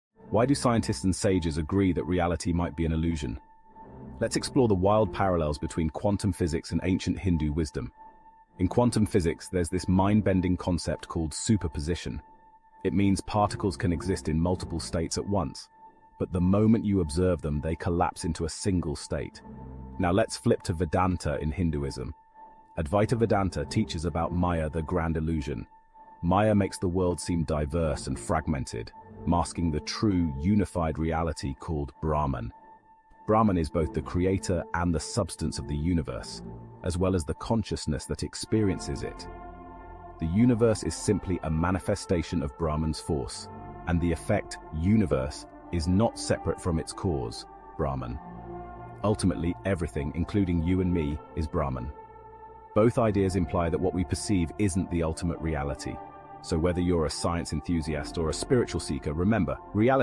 Quantum superposition and the Vedantic idea of Maya in ഹിന്ദുയിസം
Quantum physics' concept of superposition and the Vedantic idea of Maya in Hinduism share intriguing parallels. In quantum mechanics, superposition refers to the ability of particles to exist in multiple states simultaneously. However, when observed, this superposition collapses into a single state, implying that our observation shapes reality. Similarly, in Advaita Vedanta, Maya is the illusion that veils the true, non-dual nature of the universe, Brahman, and presents the world as diverse and fragmented. Maya creates the perception of separation between objects, people, and experiences, much like how the collapse of superposition presents a specific reality.